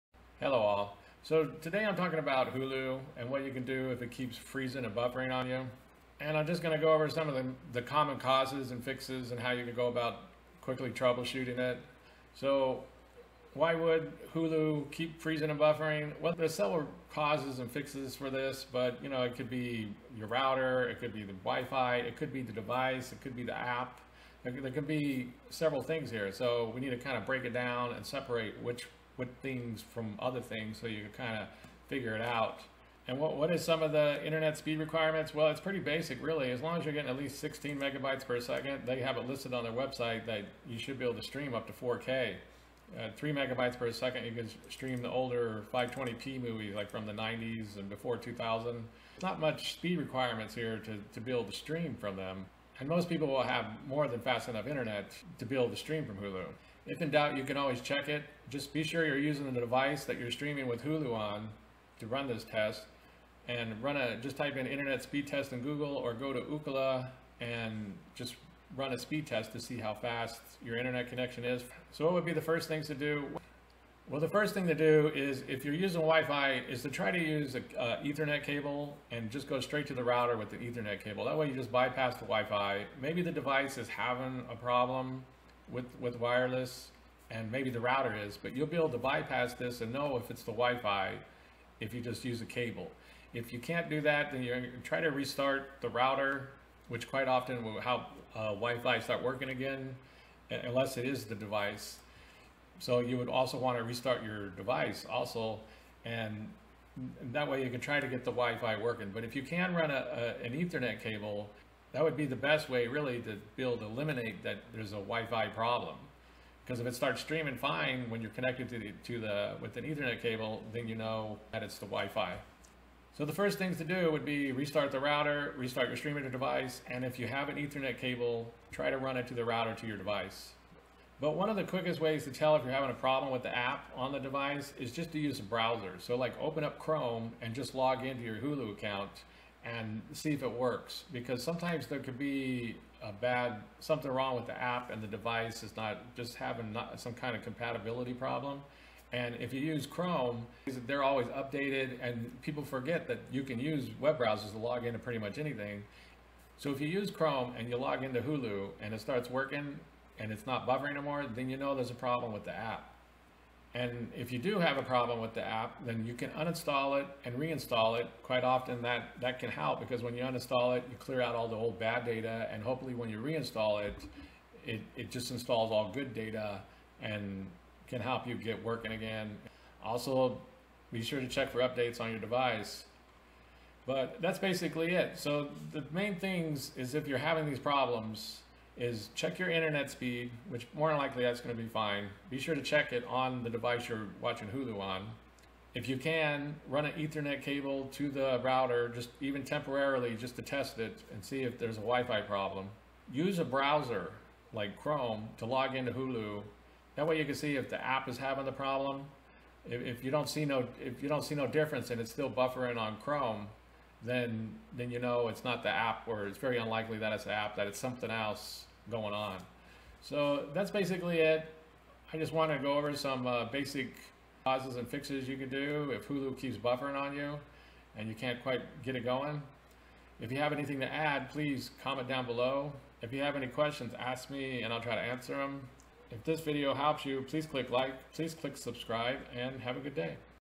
Causes and Fixes when Hulu Keeps Freezing and Buffering
How to troubleshoot what is causing Hulu to buffer or freeze.